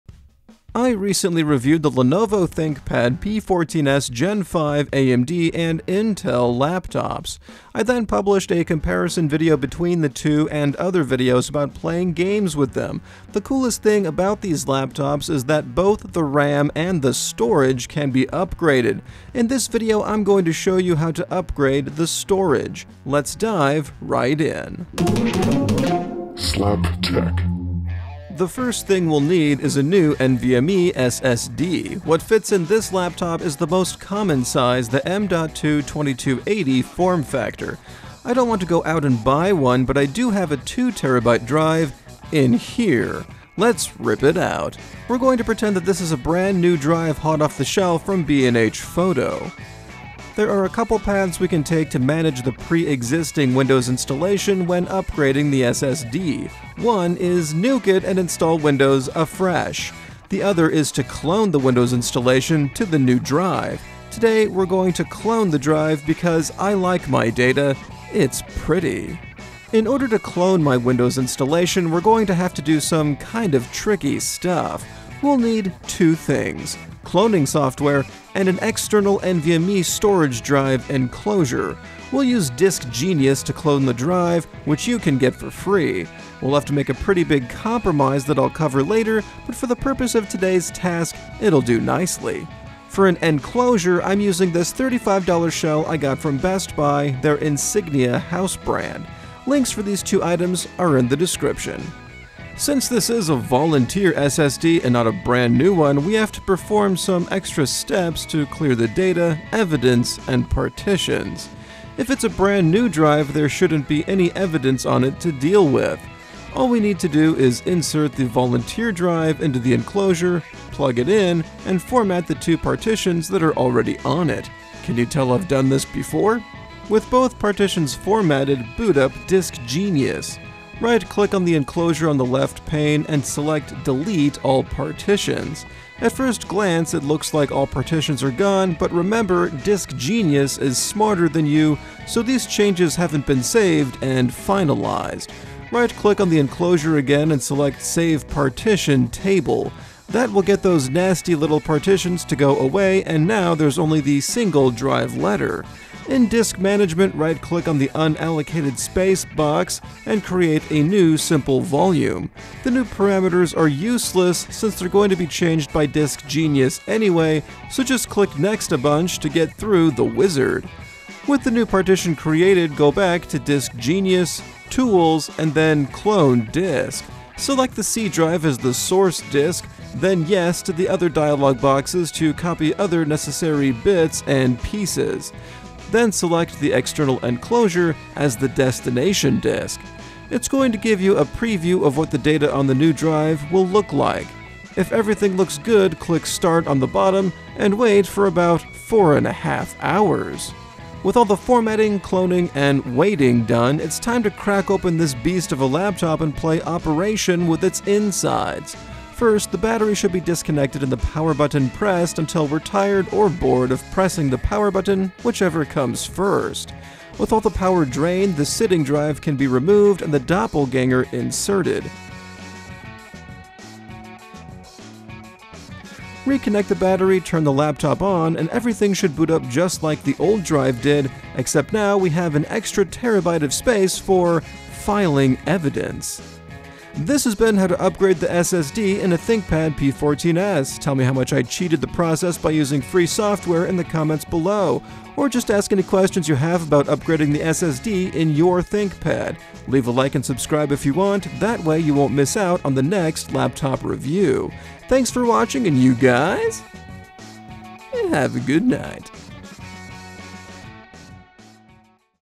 How To Upgrade the SSD In Your ThinkPad | Slap Tech
Including a full tutorial on how to CLONE your current Windows installation!

Check out Joel Michael's comparison of the ThinkPad P14s Gen 5 AMD and Intel versions here: